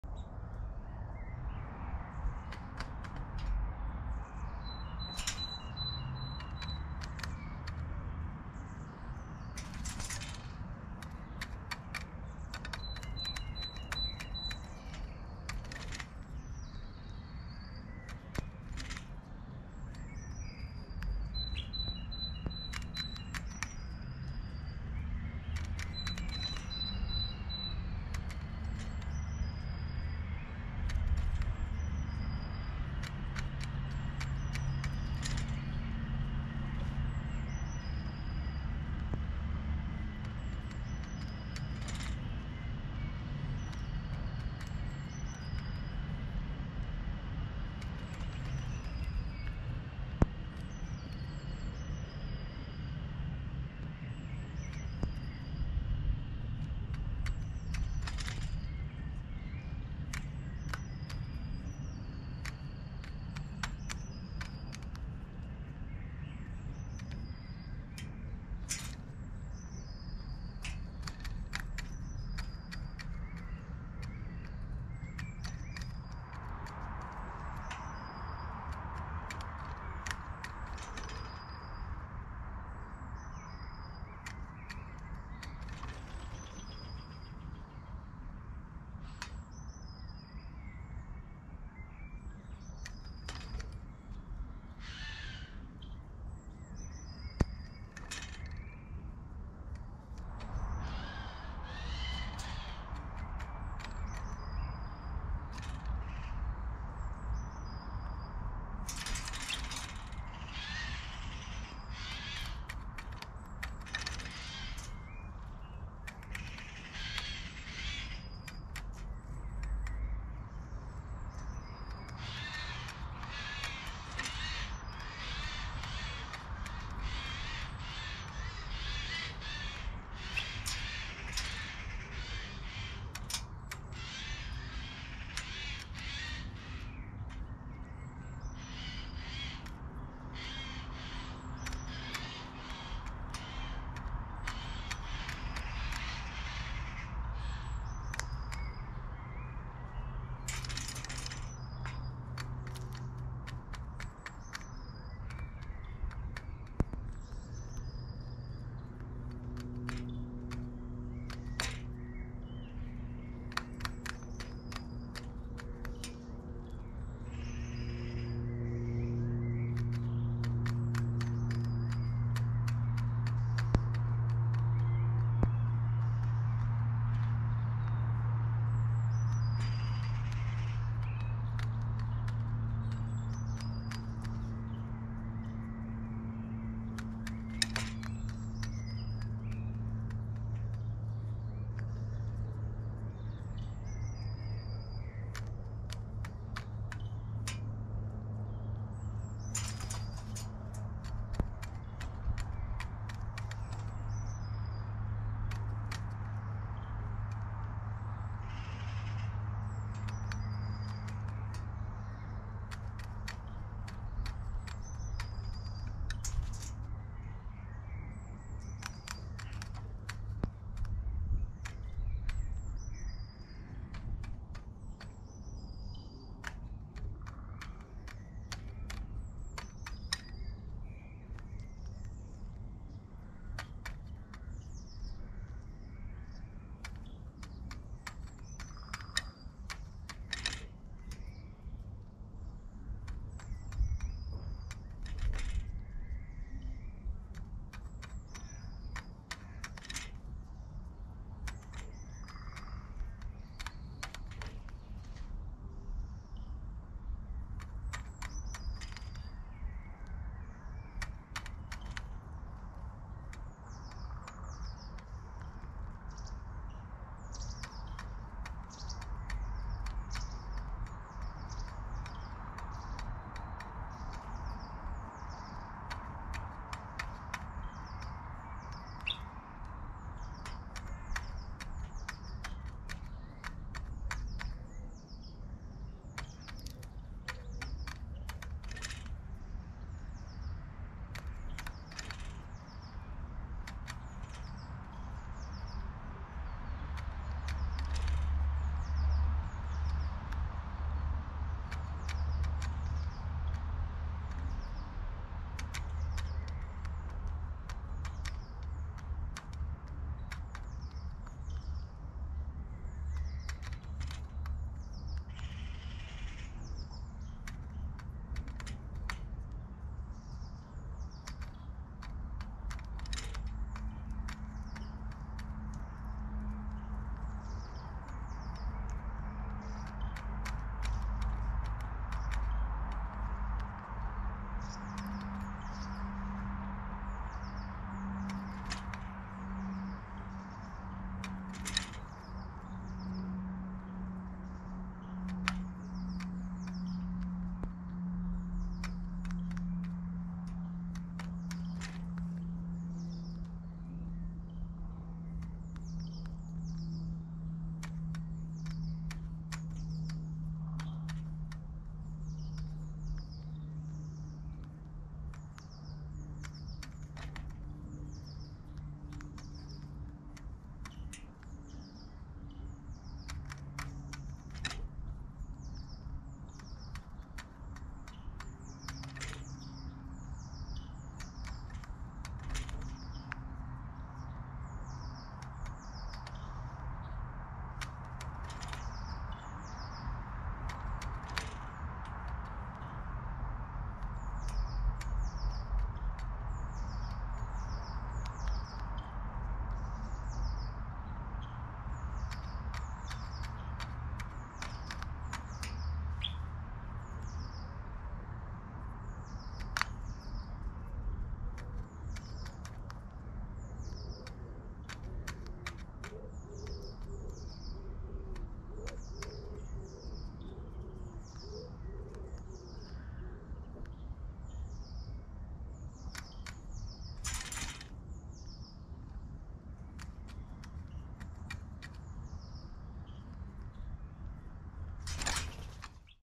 Birdwatching - Magpie at bird feeder GoPro 9 4K
Filmed with GoPro 9 on motion detecting (GoPro Labs)